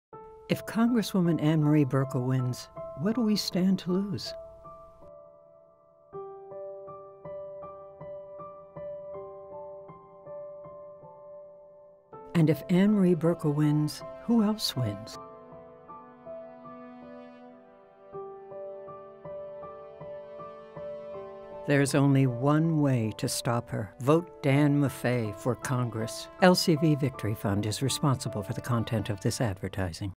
If Buerkle Wins, What Do We Stand To Lose?
A vote for Ann Marie Buerkle is a vote for Big Oil, Sarah Palin and Rush Limbaugh -- if they win, New Yorkers lose.